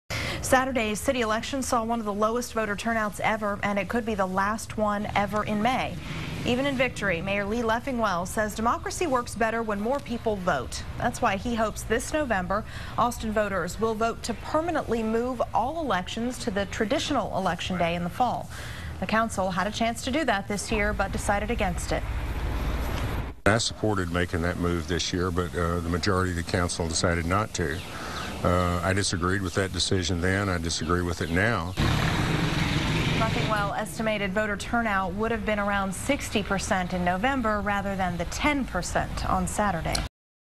Low turnout for city election - 6 pm News
Even in victory, re-elected Mayor Lee Leffingwell says democracy works better when more people vote.